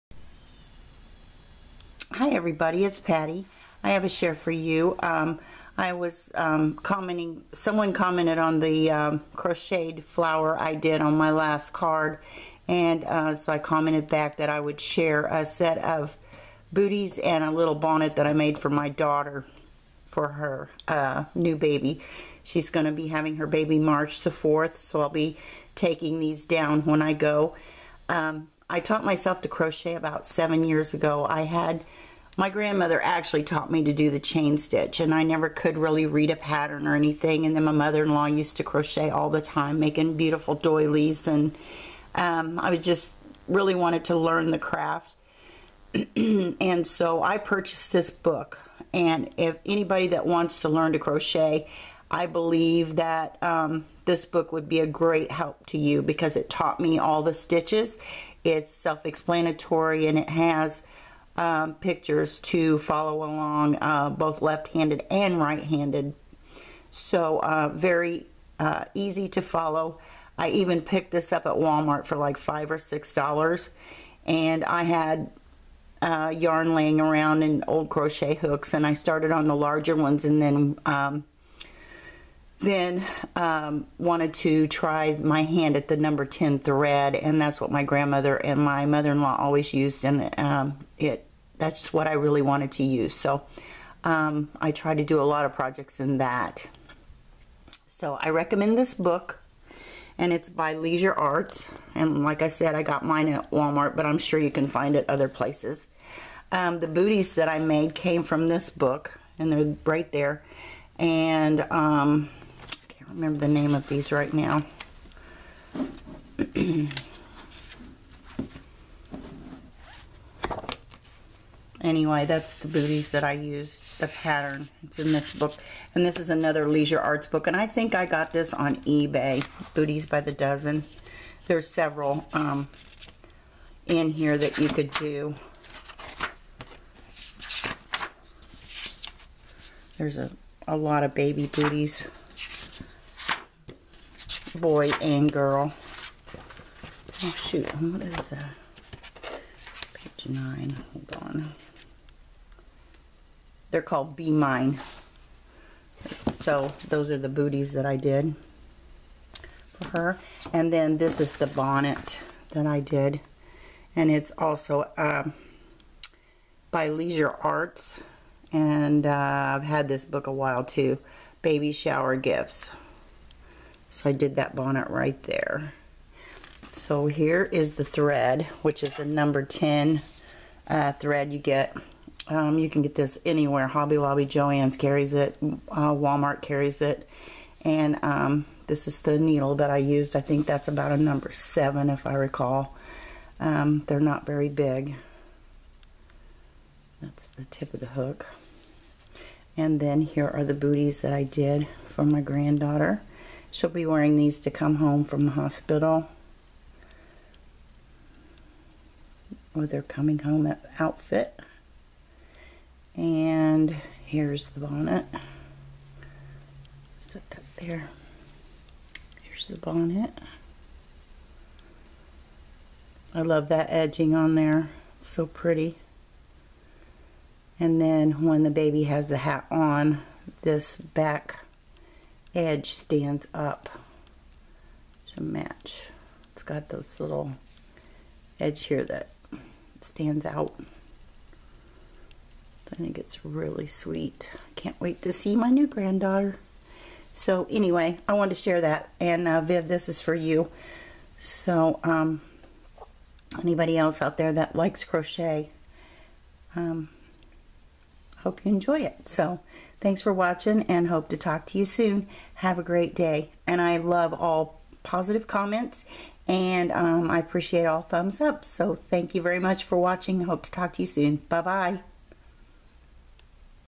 Crocheted Baby Booties & Bonnet
I share crocheted Baby Booties and A Bonnet I created for my Granddaughter. The pattern books used are given in the video, Aunt Lydia's # 10 cotton thread and a # 7 steel crochet hook were used to work the patterns.